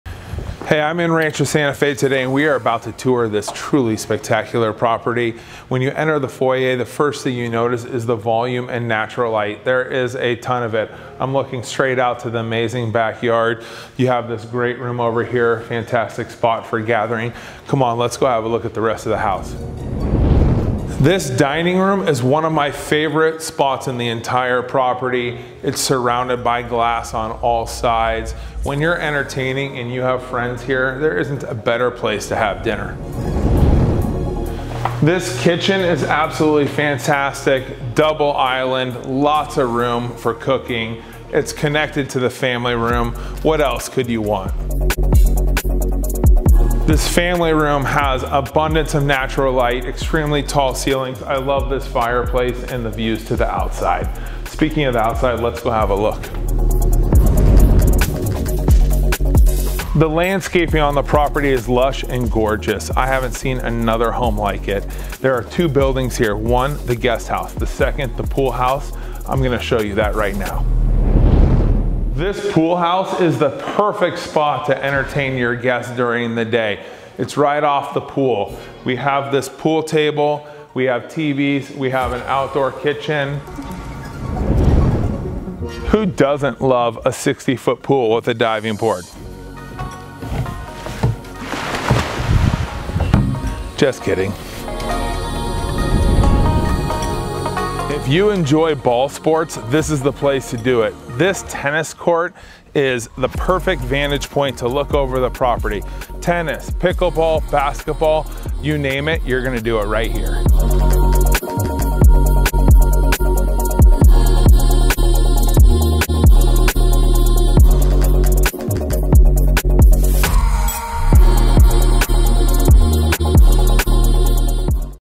Inside a $6.75 Million Dollar Private Estate | 15915 Via Pato
15915 Via Pato Rancho Santa Fe, California 92067

Located on the Westside of the Rancho Santa Fe Covenant, this single-level contemporary masterpiece offers 3.24 wooded & flowering acres. Complete with a full-sized tennis court & pavilion, 60 foot pool, 2 detached structures & garaged parking for 5 vehicles, this marvelous estate provides the best lifestyle year-round. Private & serene, this meticulous home welcomes you with lush mature landscape from the moment you arrive. The architecture & design of this home truly brings the outdoors in. The skylights, windows & doors pour natural light throughout & there are views of the beautiful gardens from every room. Complete with incredible landscape lighting, it's no surprise this home was the winner of the Presidential Award for Landscape Design. This property features approximately 8,545 square feet of livable space spread amongst the main home & two detached structures. The 6,775 square feet main home offers 4 bedroom suites, a beautiful office, chef's kitchen, formal living & dining rooms, private wine tasting room, temperature controlled cellar for 292 bottles & an attached 3 car garage with oversized doors. The detached 1 bedroom/1 bathroom guest house has an attached 2 car garage. The spacious pool house is the perfect place to entertain with the pool table, bar & kitchen. Complete with a full bathroom, steam shower, room for your exercise equipment & a small office, this pool house has it all and more! The backyard is what makes this home unique & one-of-a-kind. The craft house built into the play structure is every child's dream come true! Swim laps in the 60 foot pool with separate jacuzzi or perfect your swing in the full sized tennis court with pavilion. With multiple patios & areas to dine al-fresco, this property gives you every excuse never to leave home! Just minutes from the best beaches, award winning schools, shopping, restaurants & easy freeway access, this location cannot get any better. Prepare to be amazed!